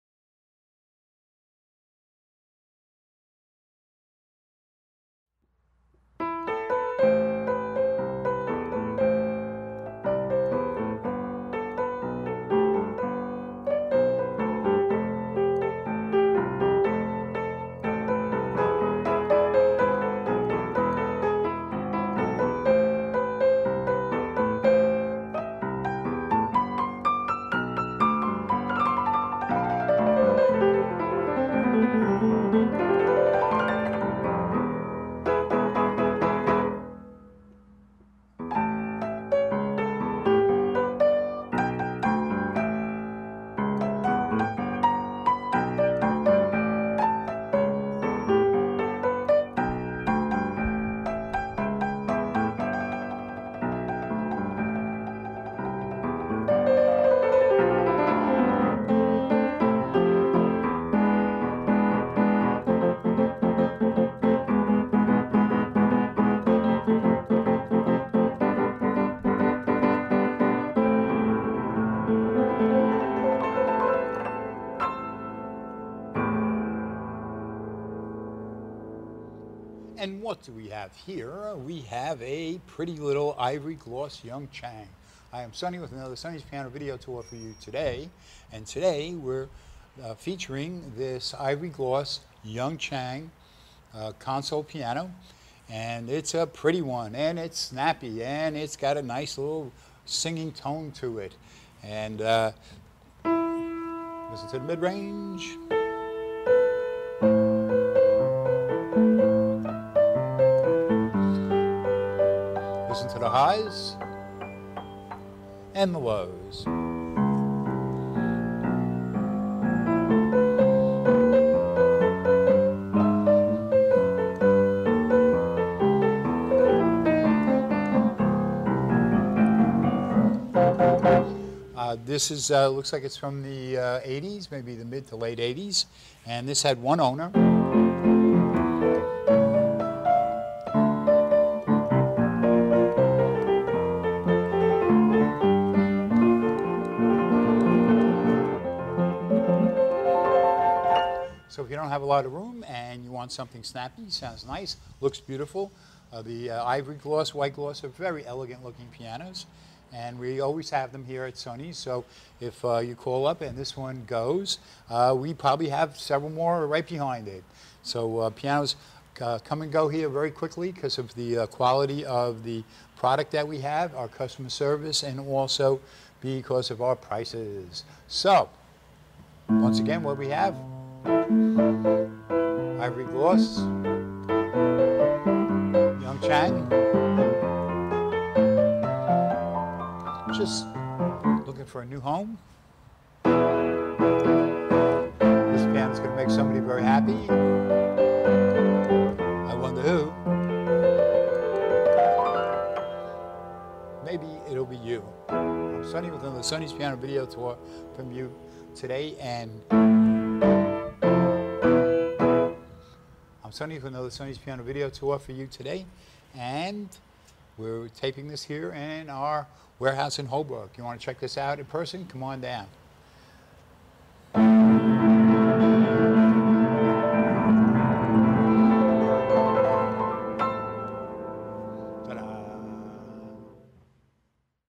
White Gloss Upright Console Piano Young Chang $1750.
Nationwide shipping!
Pretty White Gloss Console Young Chang Piano, low mileage, pristine, plays and looks great. One previous owner. Made in 1997. 42" tall, with matching bench. Local LI/NYC/CT/NJ delivery only. Warranty. (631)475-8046